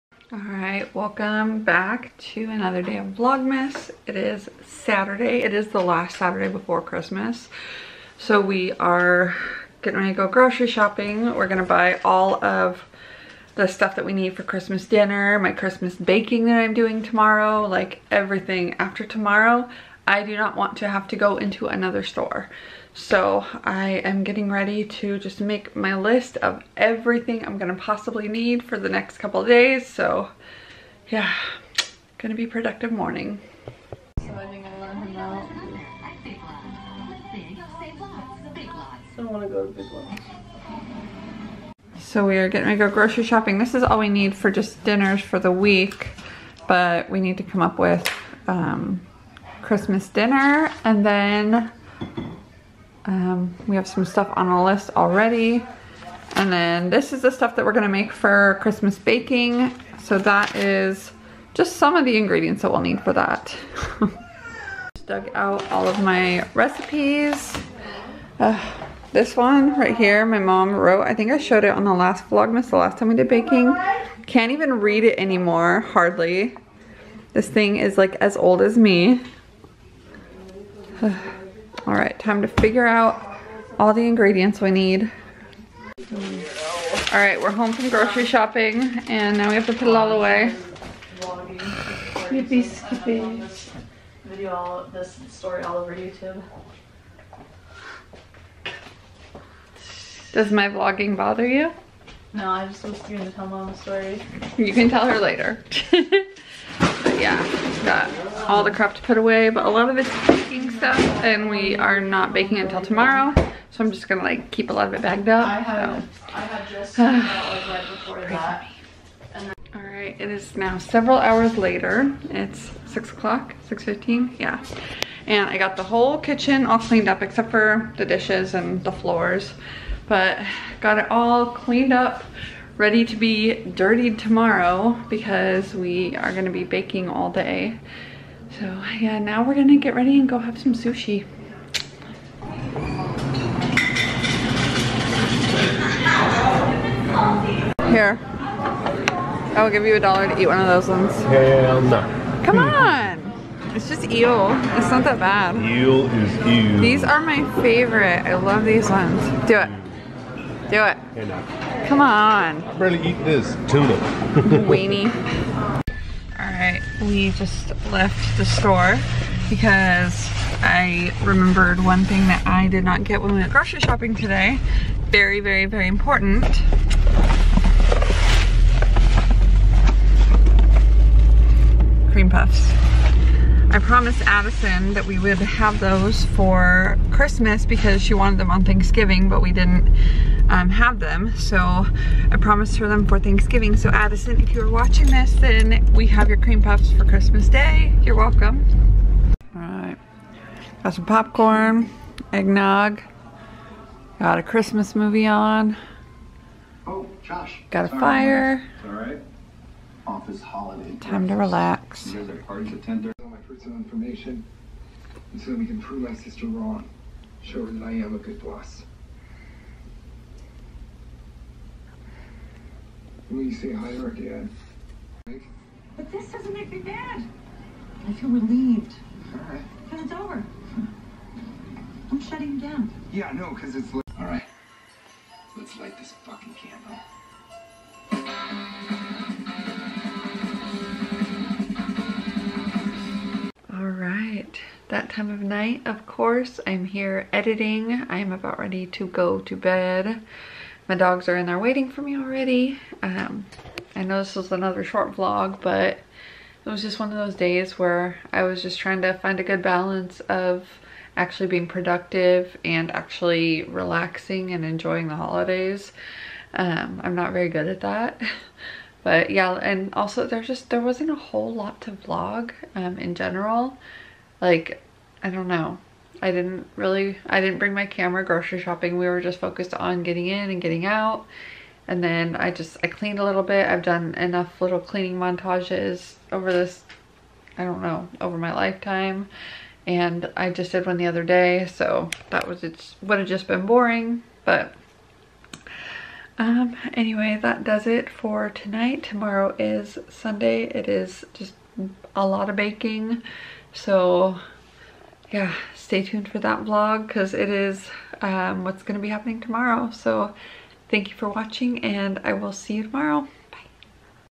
Trying to relax but also be productive | Vlogmas Day 22
Hey everyone! I hope you enjoy Vlogmas Day 22, even if it is very short. I didn't have a lot to vlog.